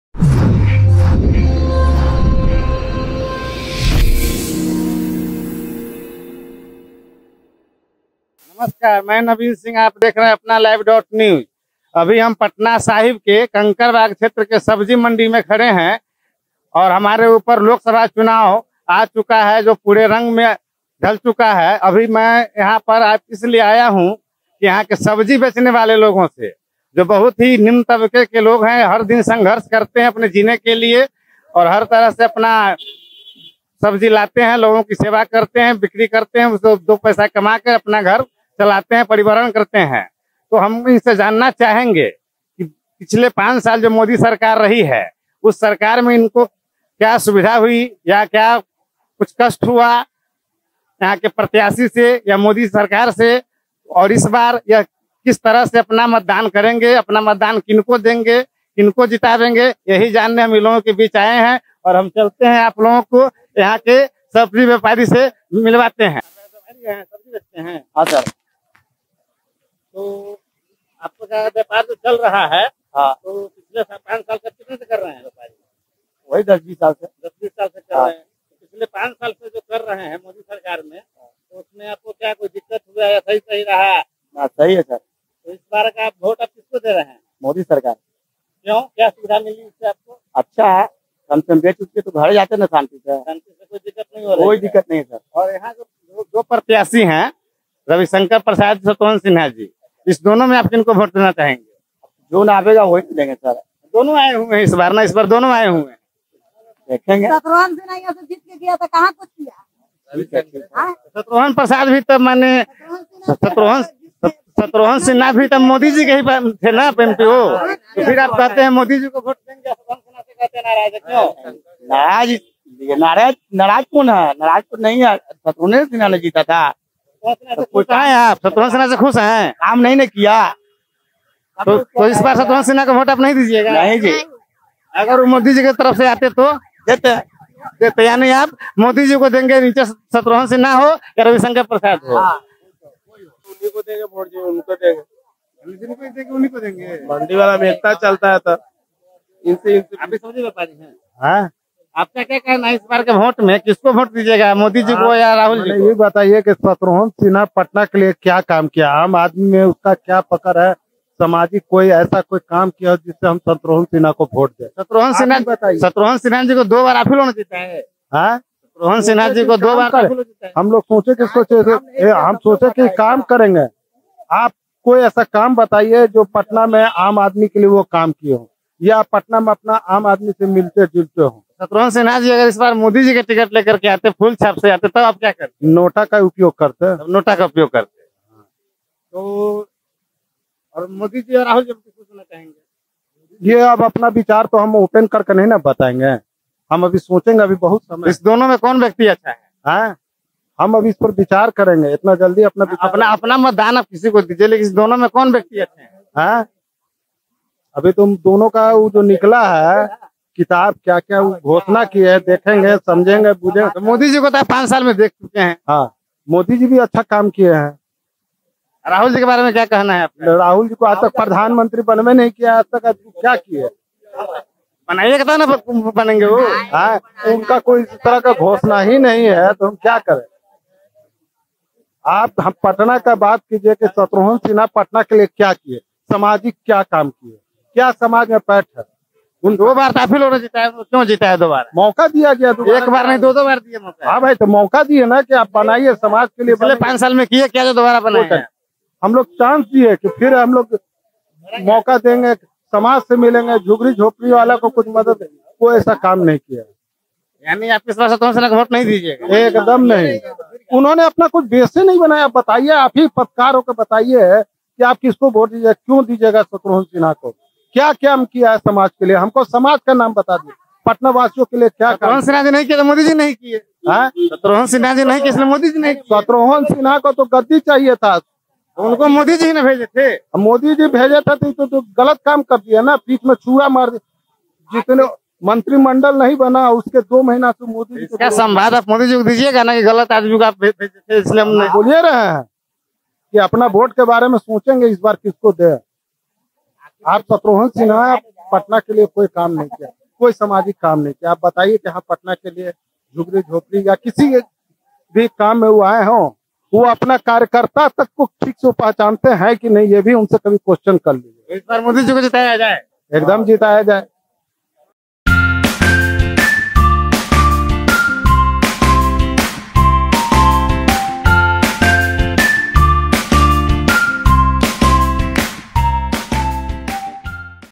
लोकसभा चुनाव 2019 पटना साहिब सब्जी विक्रेता की पसंद शत्रुघ्न सिन्हा V/S रविशंकर प्रसाद
Voice: Naveen Singh